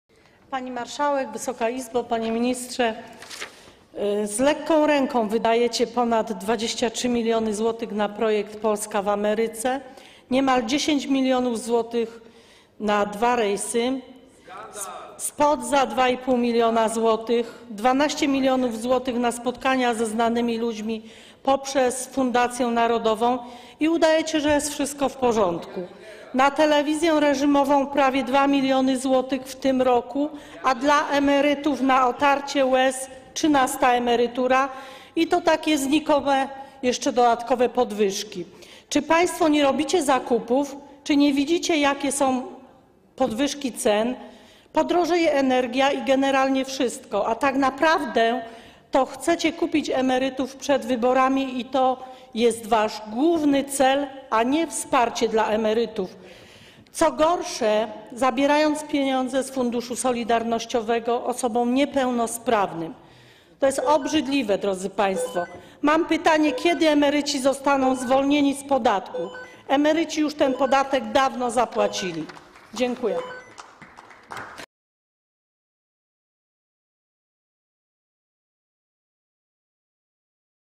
Małgorzata Pępek - wystąpienie z 8 stycznia 2020 r.
Popierasz? Udostępnij! Małgorzata Pępek - wystąpienie z 8 stycznia 2020 r.
3. posiedzenie Sejmu IX kadencji

Pierwsze czytanie rządowego projektu ustawy o dodatkowym rocznym świadczeniu pieniężnym dla emerytów i rencistów